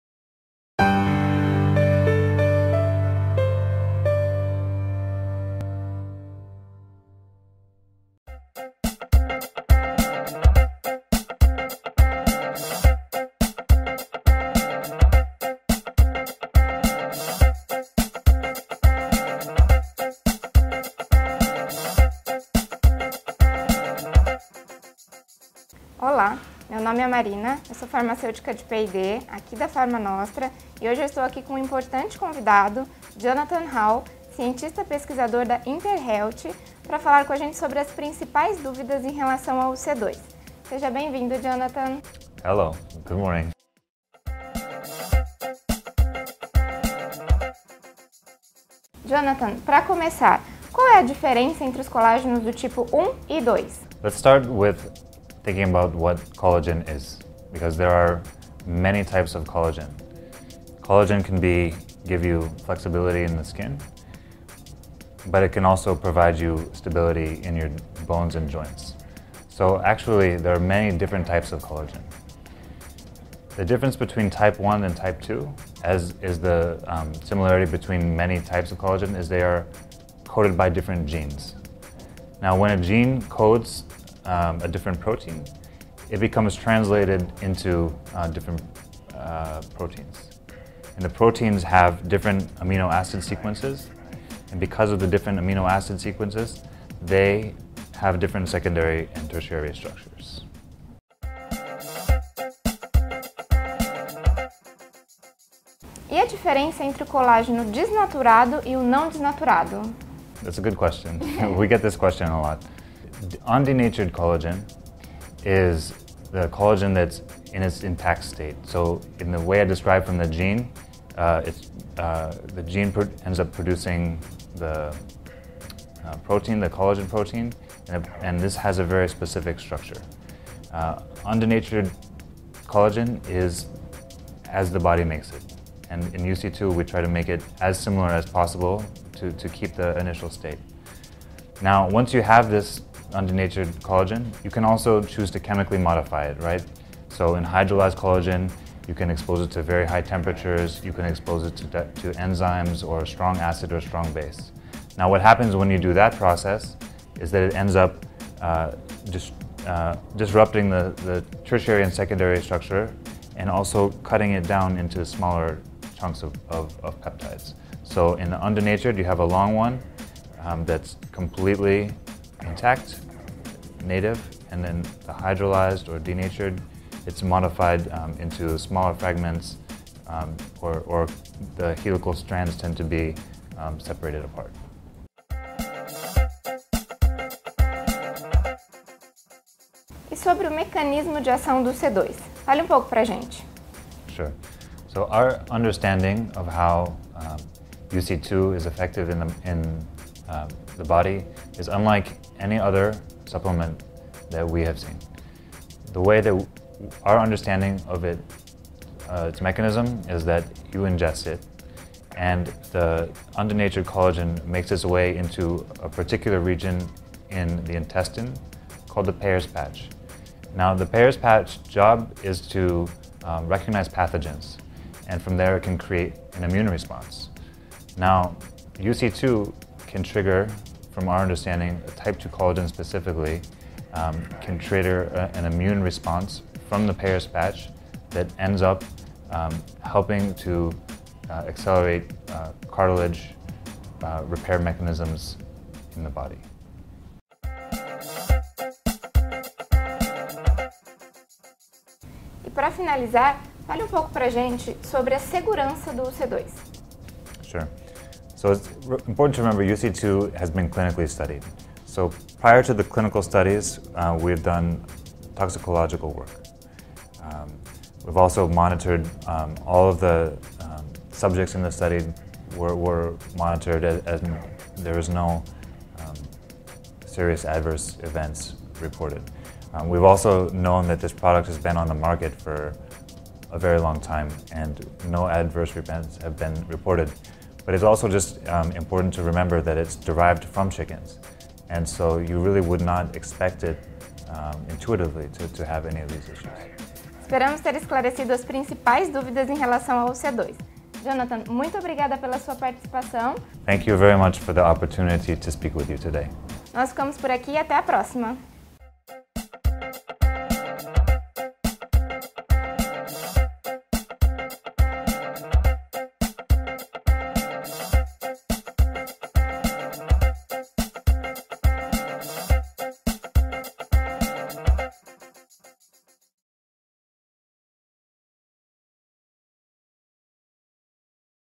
Conexão PharmaNostra UC ll Conheça os Benefícios
Entenda mais sobre os benefícios do UC ll.
Marina Jordão Delfine, Farmacêutica de Pesquisa e Desenvolvimento PharmaNostra; entrevista Jonathan Hall; Cientista Pesquisador da InterHealth.

O último estudo com UC-II® conduzido pela InterHealth, ganhou o primeiro lugar na 10ª Conferência Anual de Suplementos Nutricionais de 2013, da Scripps Center for Integrative Medicine, na Califórnia (EUA).
Foi comprovado que UC-II® apresenta excelentes resultados em indivíduos saudáveis, praticantes de esportes, aliviando as dores e tempo de duração do desconforto após o exercício, com melhora da mobilidade e flexibilidade.

UC II está disponível na PharmaRav Farmácia de Manipulação